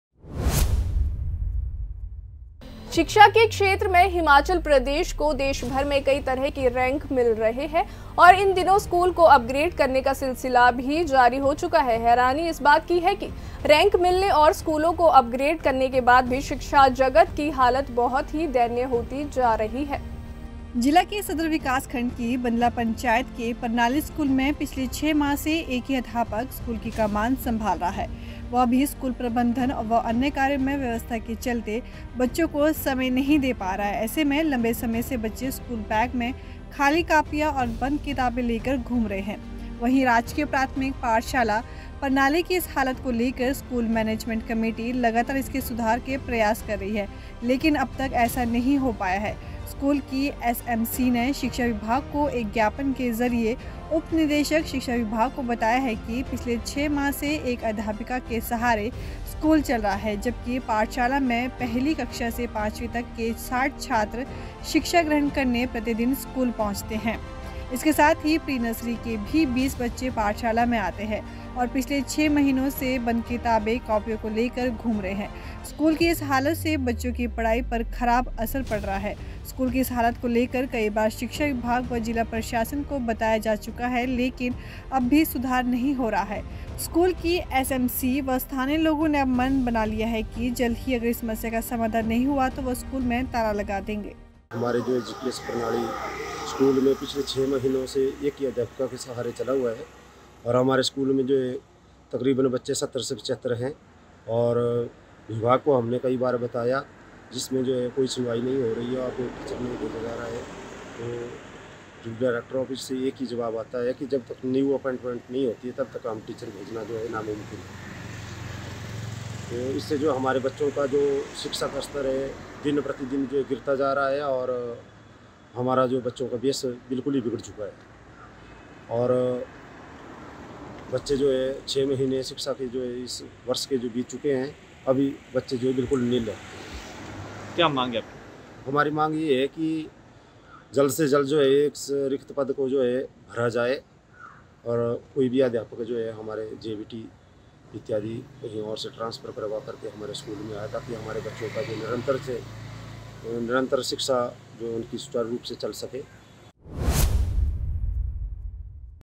छह महीनों से खाली कापियां लेकर घूम रहे परनाली स्कूल के बच्चे | Himachal Today
छह महीनों से खाली कापियां लेकर घूम रहे परनाली स्कूल के बच्चे
Children of Paranali school roaming around with empty copies for six months